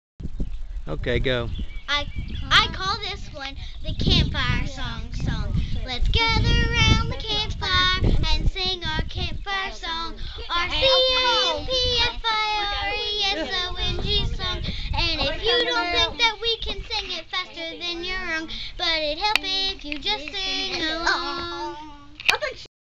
Silly Song
A silly song sung by Reagan & Casey on Easter Day 2009.